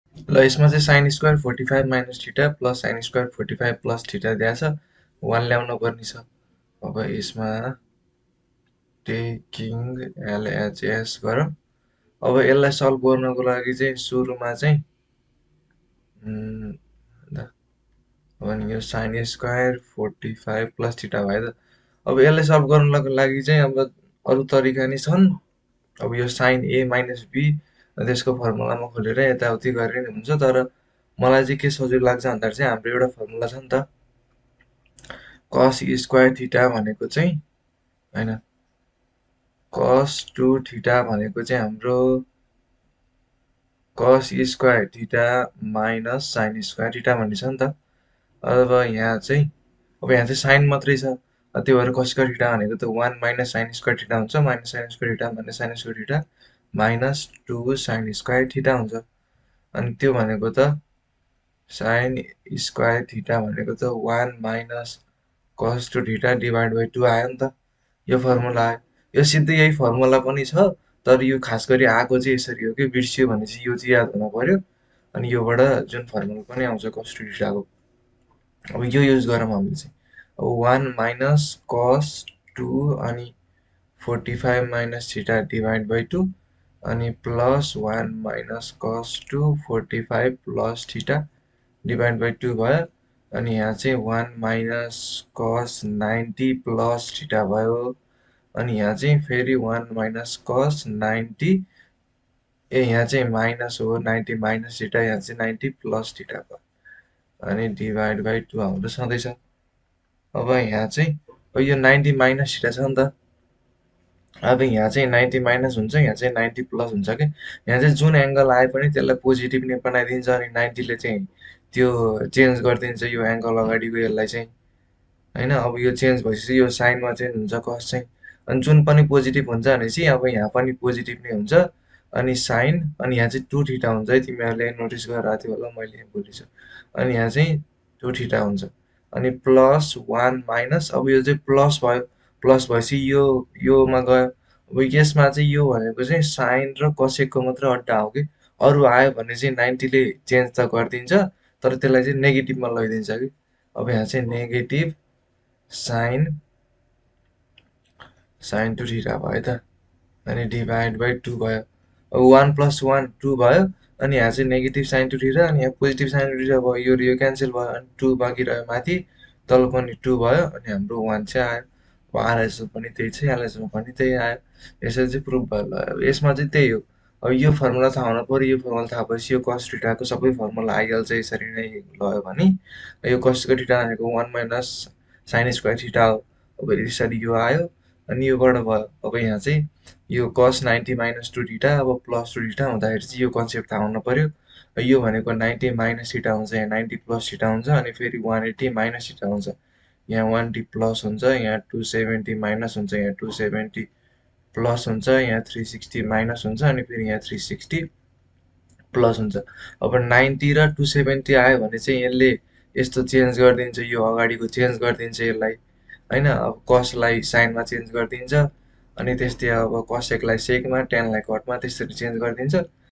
Prove sin^2(45-theta) + sin^2(45+theta) = 1
Dibas, a creator of the SEE Ko Guide, is walking through the solution to the trigonometry question of the optional math subject of class ten.

We have published 10 educational apps on the Play Store. All of the apps are available for free.